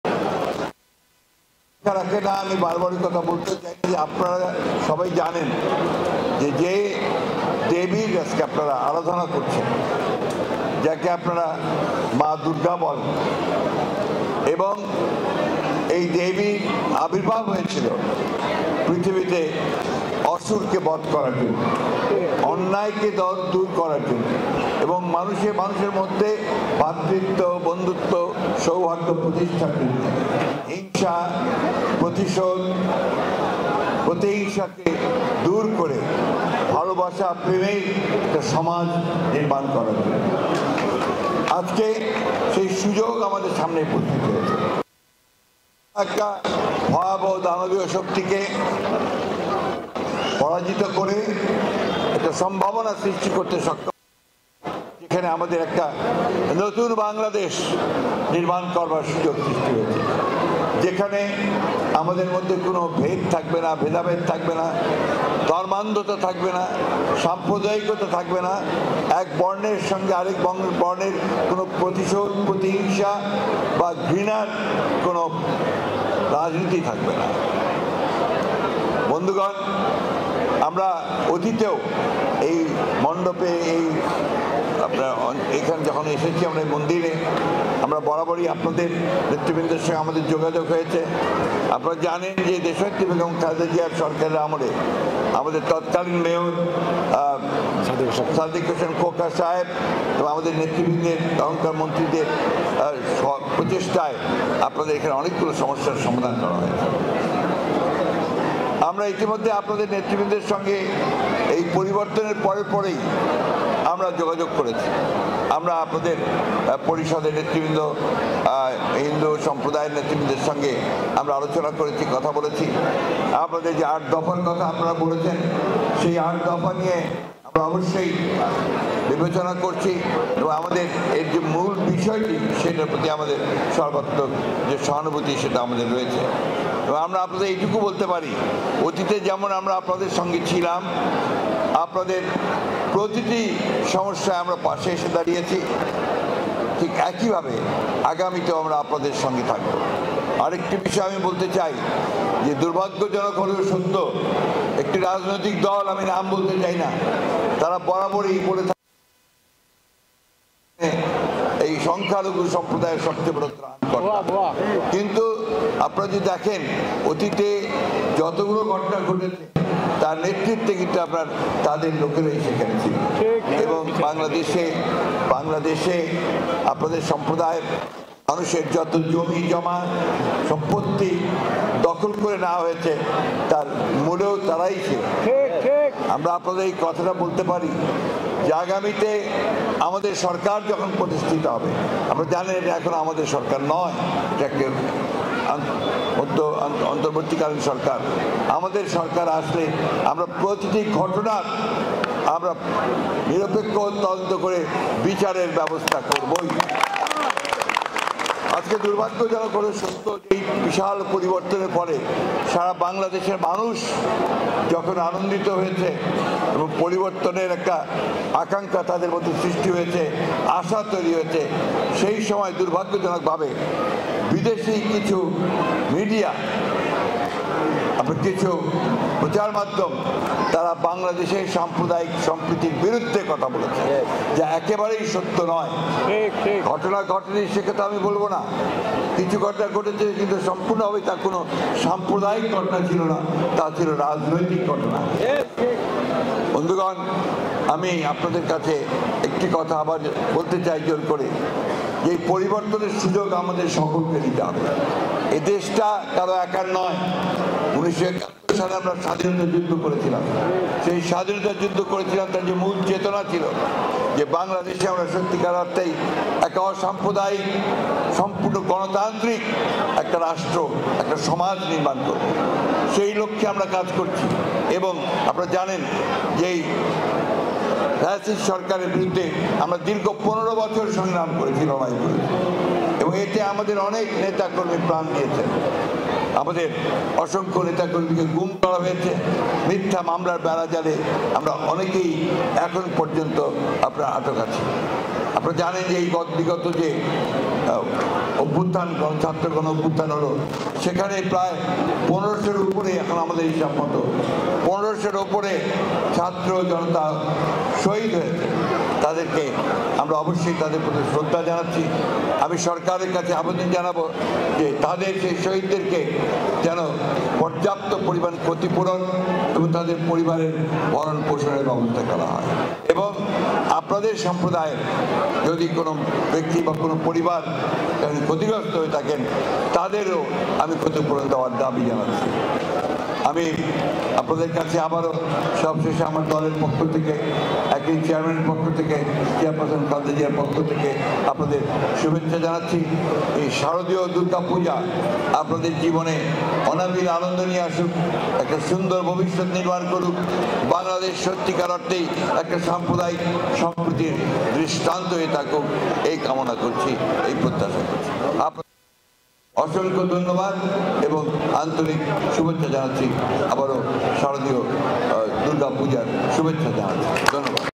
ঢাকাশ্বরী মন্দির পরিদর্শন শেষে: মির্জা ফখরুল ইসলাম আলমগীর বক্তব্য
ঢাকেশ্বরী মন্দির পরিদর্শন শেষে মিজা ফখরুল ইসলাম আলমগীরের বক্তব্য